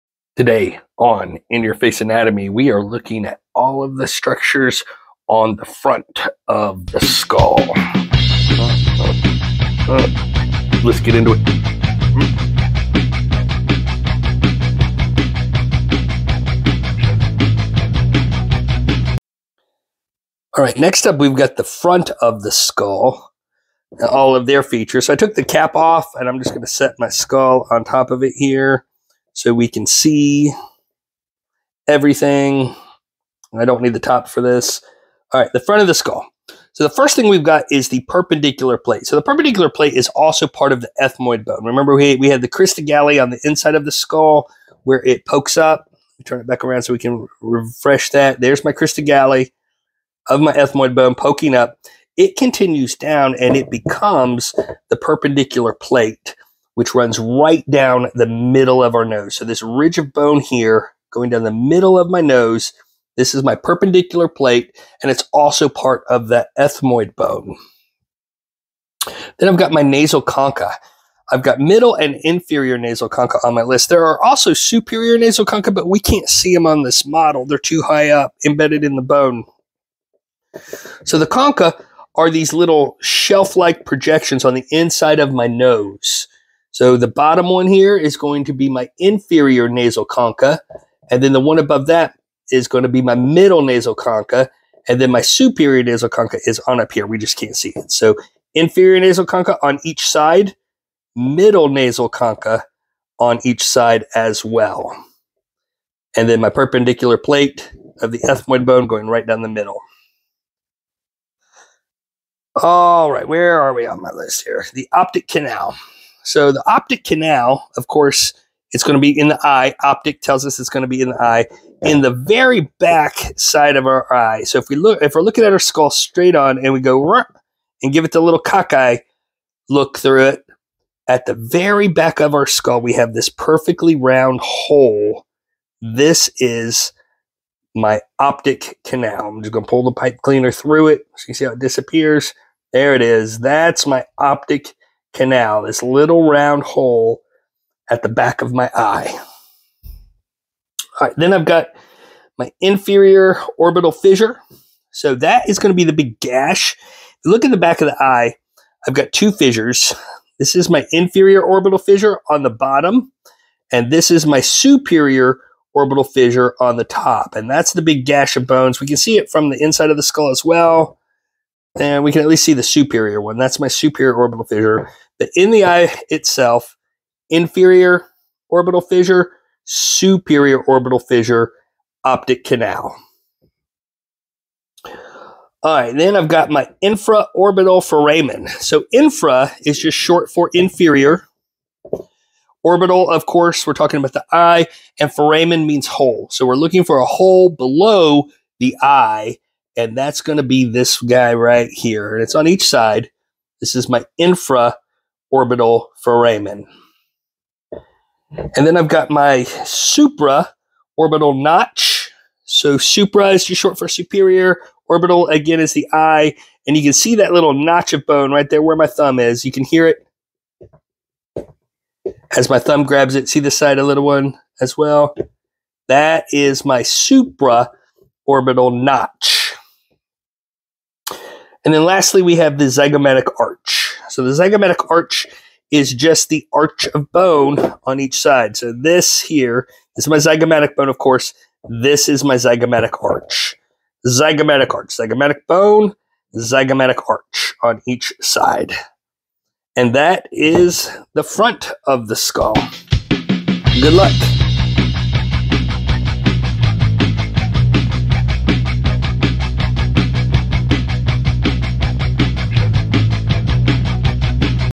Ch 6 The Skeletal System (Front of the Skull)
In this video we delve into the boney features on the front of the skull.